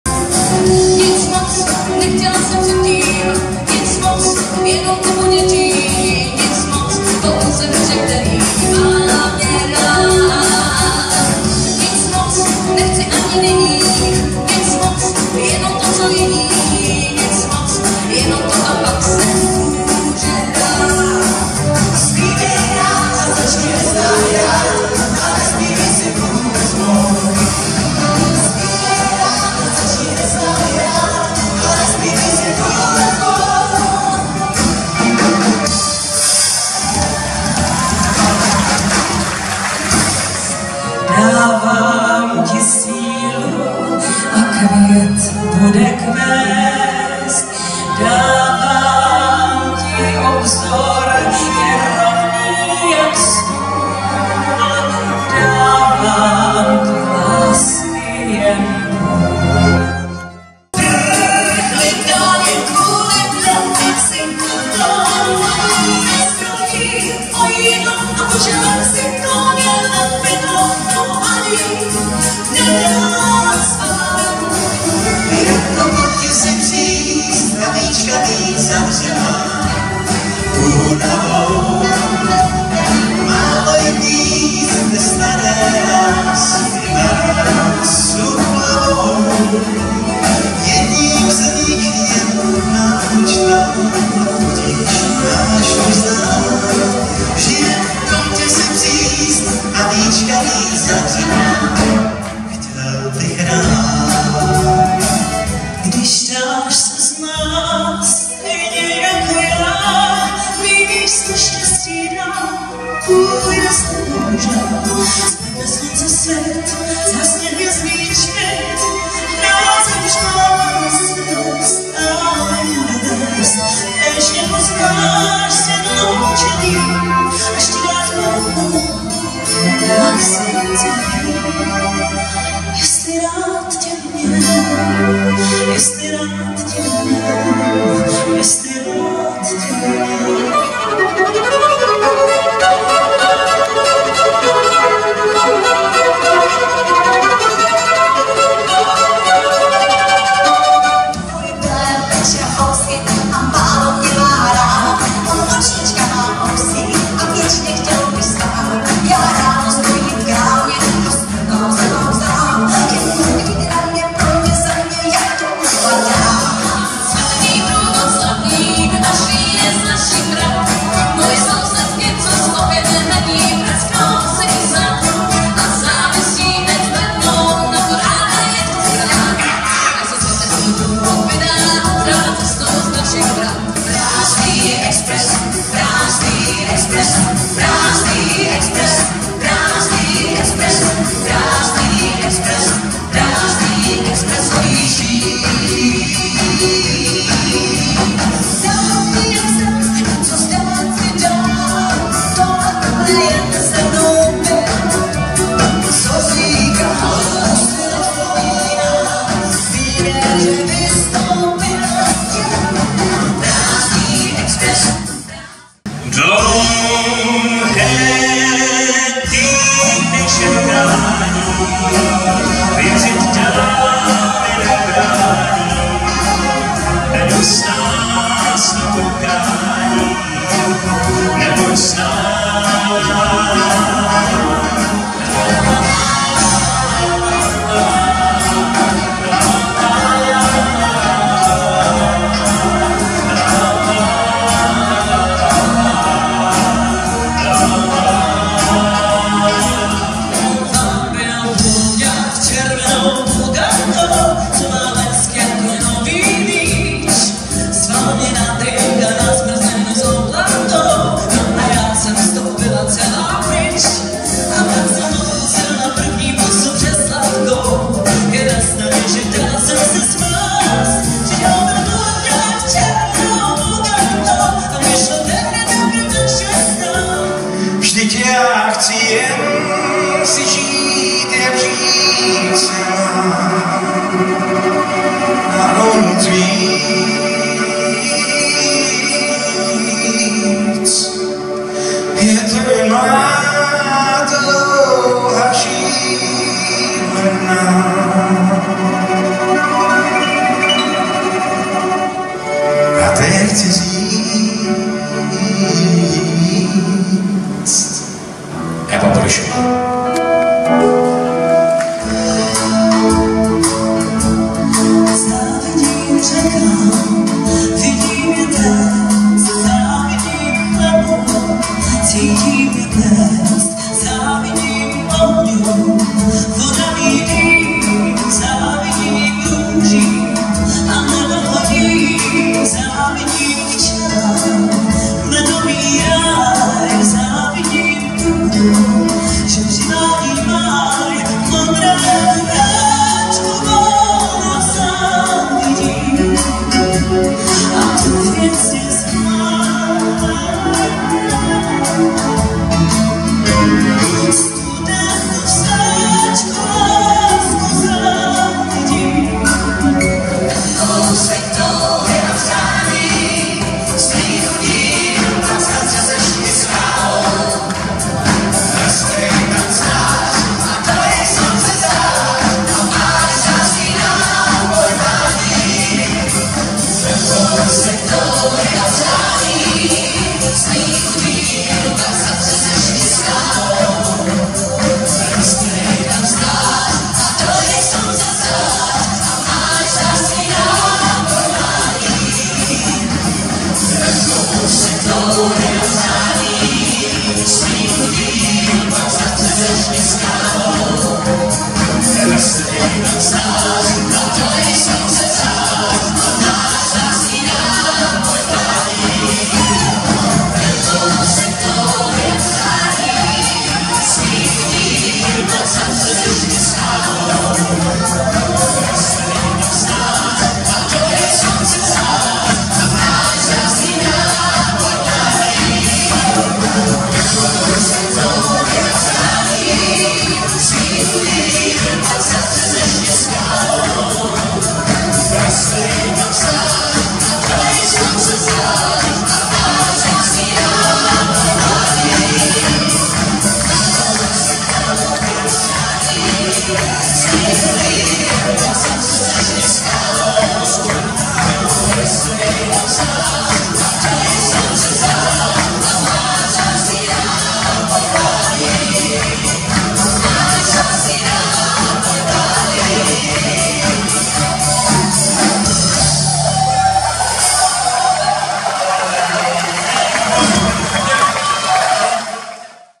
Vzpomínkový koncert na Naďu Urbánkovou - Eva Burešová, Michal David, Pavel Šporcl a další
Moondance Orchestra se sborem a Epoque Quartet, Martin Kumžák